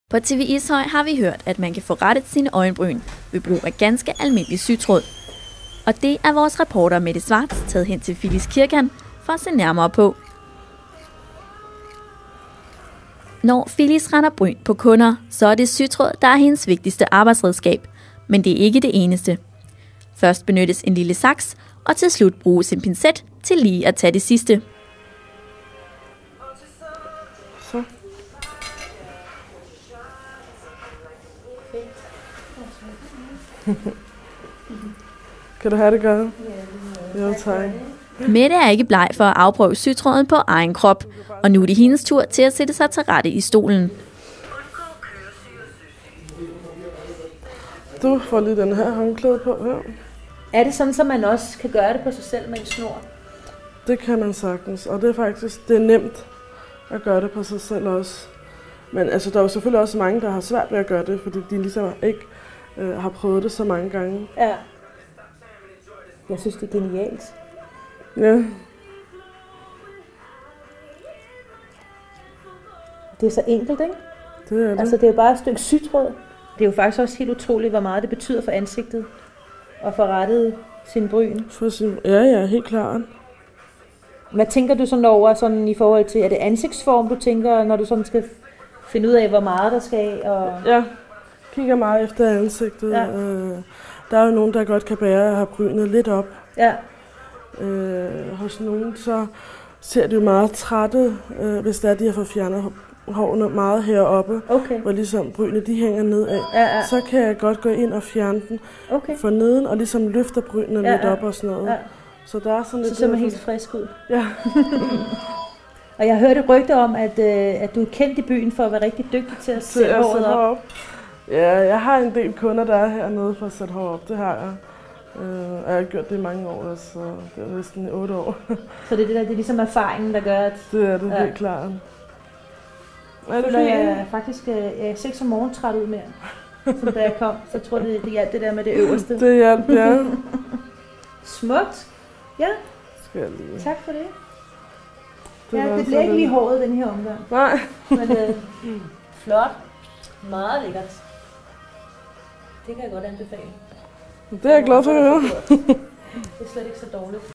Øjenbryn rettet med sytråd - TV-Ishoj
Hos frisør Filiz Kirkan kan man få rettet sine øjenbryn med sytråd. Det måtte tv-Ishøjs reporter prøve af.